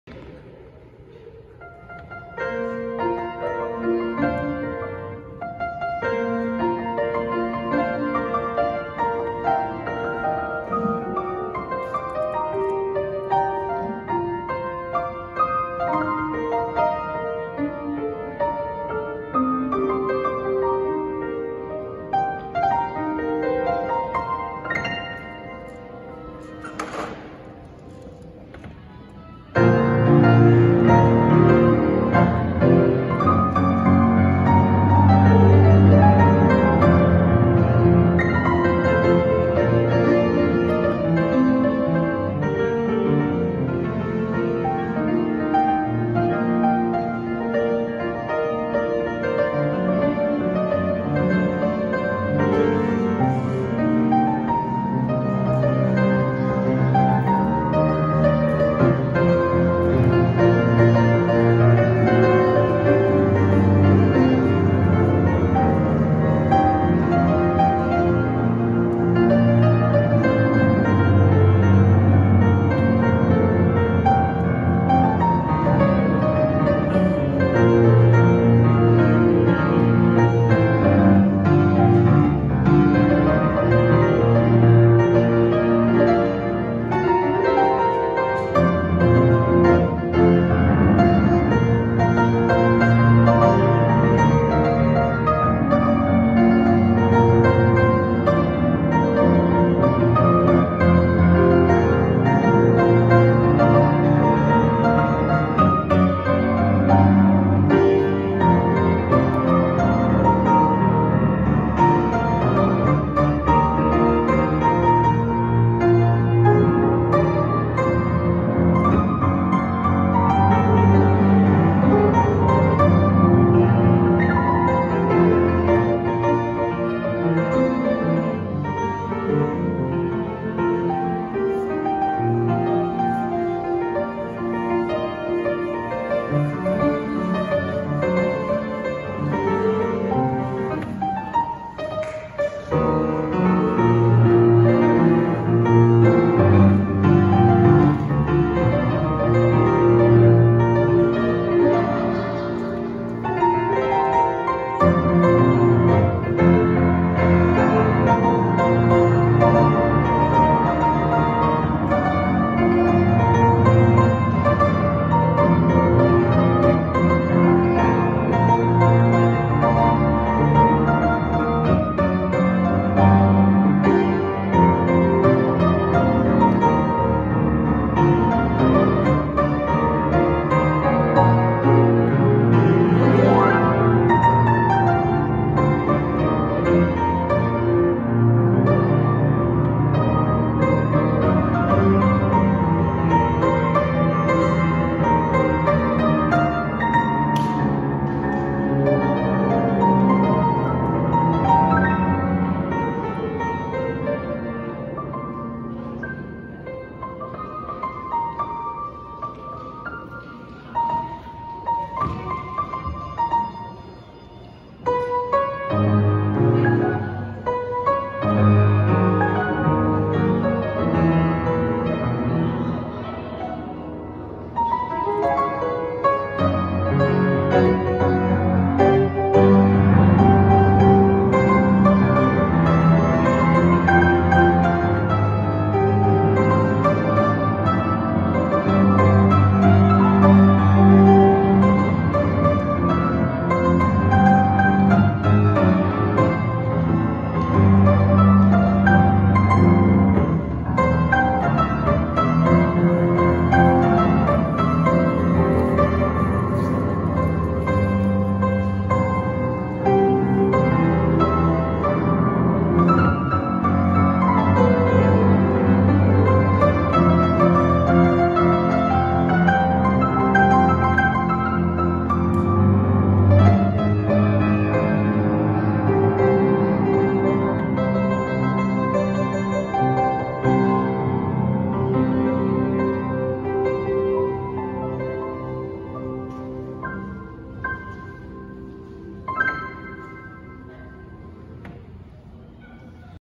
2022.3.20 ストリートピアノat淡路夢舞台
花さじき目当てで行ったら駐車場いっぱいで夢舞台へ。
歩いていたらピアノの音！
弾くしかないでしょ。
練習してなかったから下手だったって不機嫌男子になりましたとさ。